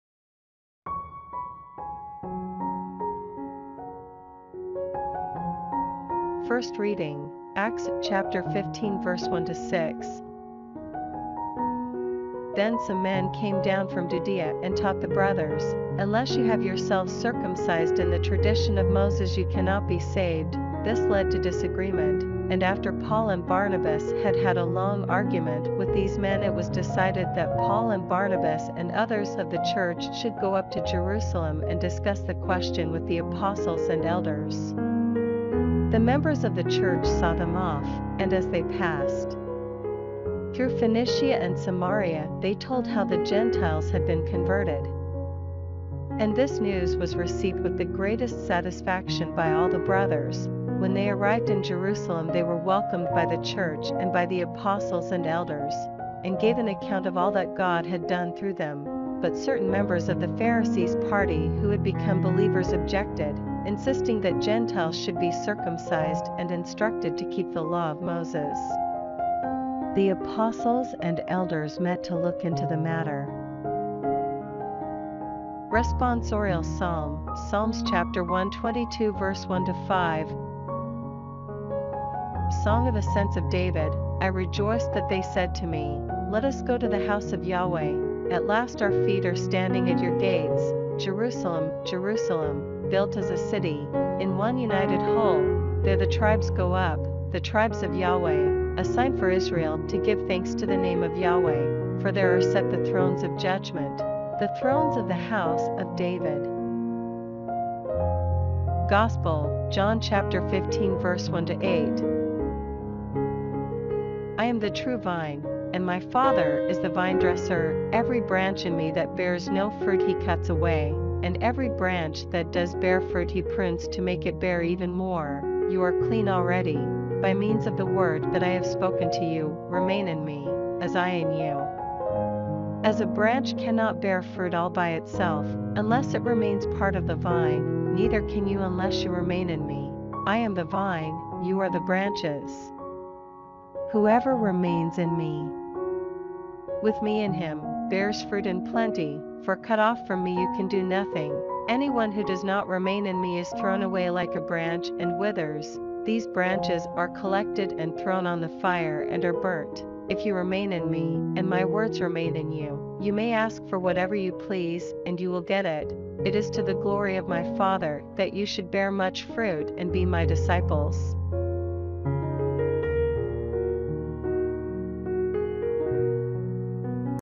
Daily Reading for Wednesday May 18, 2022
Reading 1, Acts 15:1-6
Responsorial Psalm, Psalms 122:1-2, 3-4, 4-5
Gospel, John 15:1-8